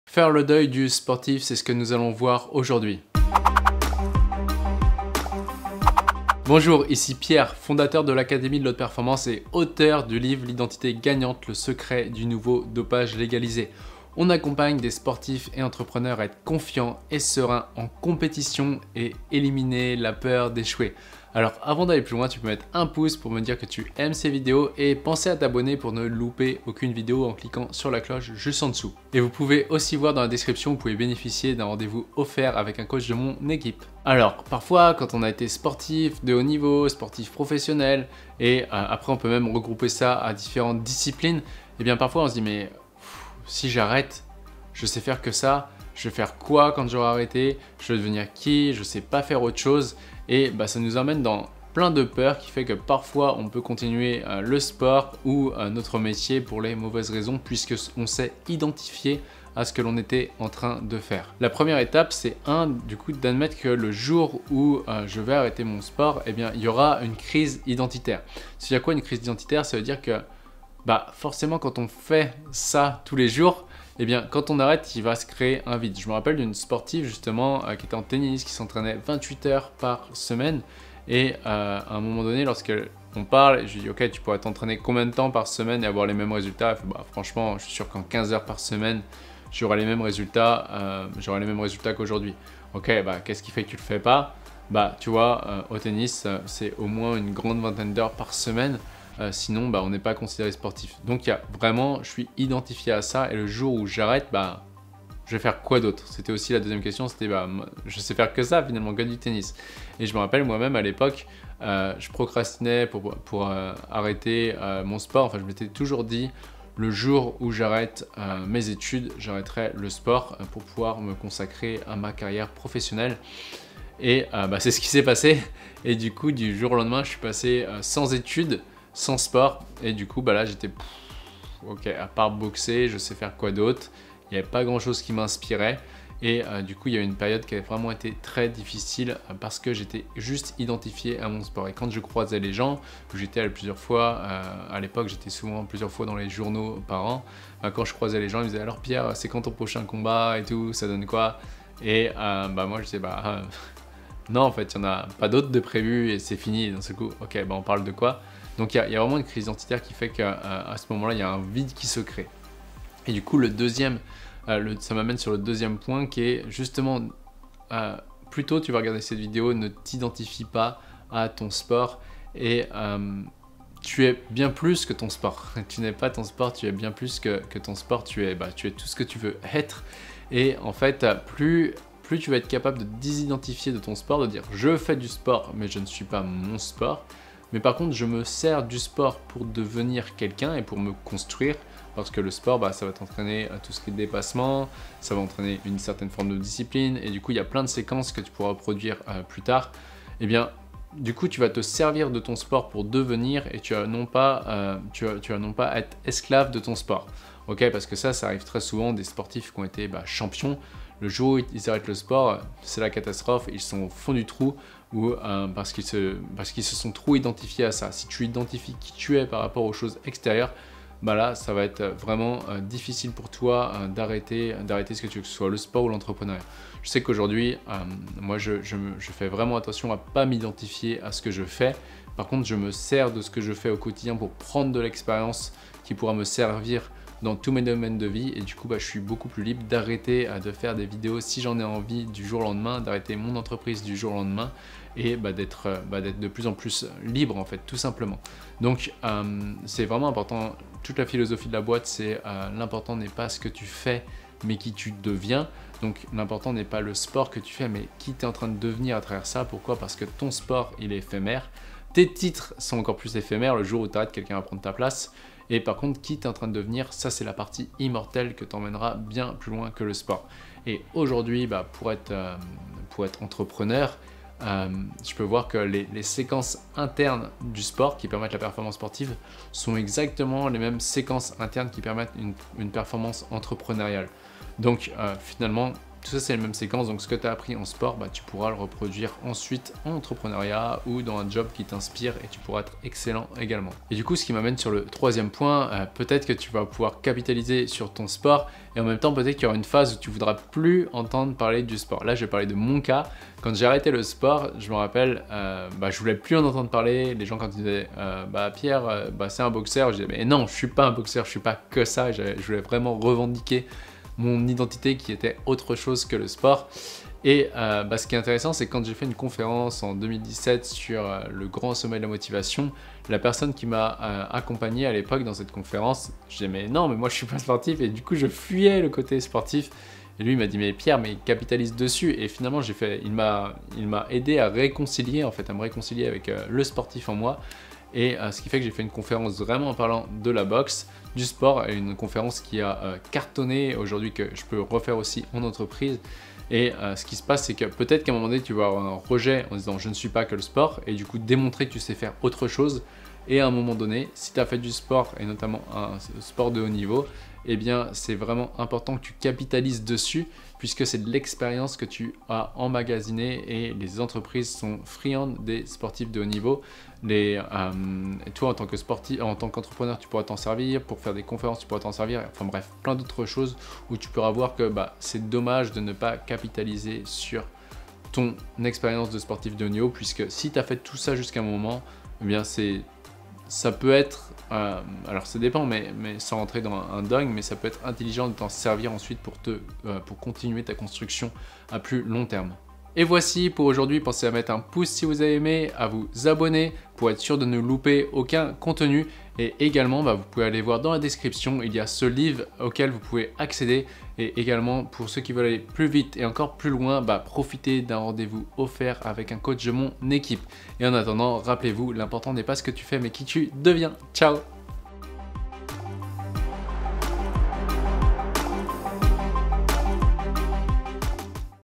Comment faire le deuil du sportif ?
Quand on est sportif de haut-niveau, on peut parfois se demander ce qu'on va faire le jour où on arrête sa carrière...

C'est souvent difficile de se projeter dans sa vie d'après car on est très identifié à ce que l'on fait.

Car lorsqu'on fait ça tous les jours, forcément quand on arrête ça crée un vide !

Une des clés pour se reconvertir au mieux (et avoir plus de bien-être et de détachement pendant sa carrière aussi) c'est de se désidentifier de votre sport.

Vous êtes bien plus que cela !

Le sport n'est qu'un moyen de devenir qui vous voulez être.

Pierre